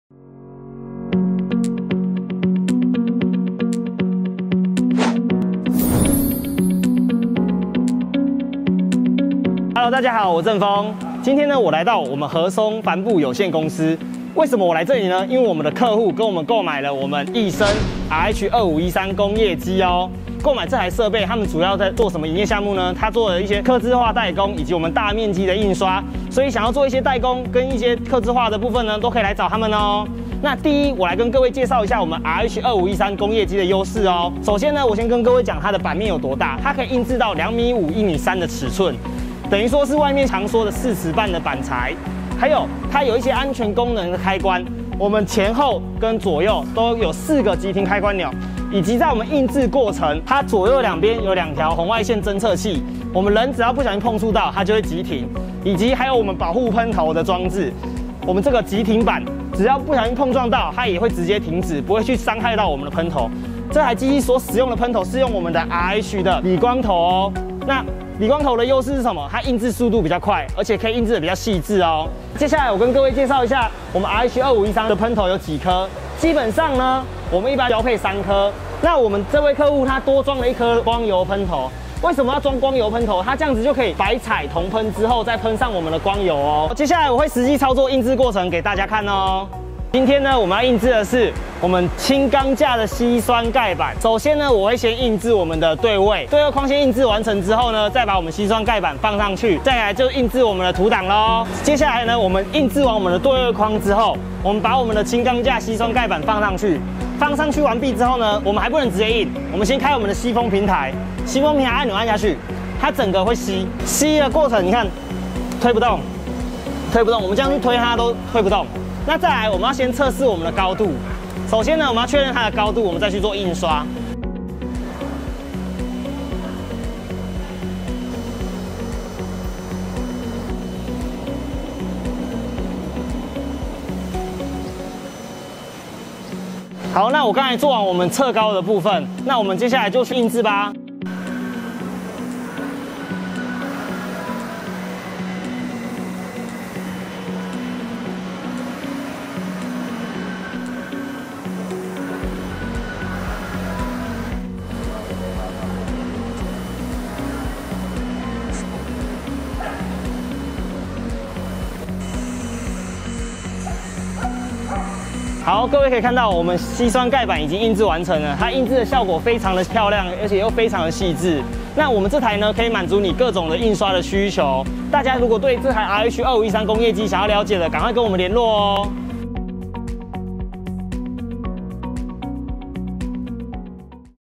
RH 2513 UV直噴工業機操作介紹 | 奕昇有限公司
🔥RH 2513 UV直噴工業機🔥 操作介紹

這次我們來到合崧帆布有限公司
合崧主要經營客製化代工、大面積材料印刷🖨
透過RH 2513 UV直噴工業機進行多材質精緻印刷👍

📣RH 2513 UV 產品特色📣
✅印製尺寸 2.5米*1.3米 (四尺半)
✅RH理光噴頭👉印製速度快、成果細緻
✅紅外線偵測器保護噴頭
✅標配3顆噴頭 可加裝光油噴頭打油光效果
✅4處急停開關鈕

📣輕鋼架矽酸鈣板 操作示範📣

1⃣印製對位框線
2⃣放置矽酸鈣板至設備
3⃣開啟吸風平台固定材料
4⃣確認噴頭高度
5⃣開始印製

用RH 2513噴印的成品顏色鮮豔、細節精緻👍
歡迎私訊洽詢賞機🙌

感謝客戶購買奕昇RH 2513 UV直噴工業機
合崧帆布有限公司
📪地址：彰化縣彰化市彰南路四段413巷166之1號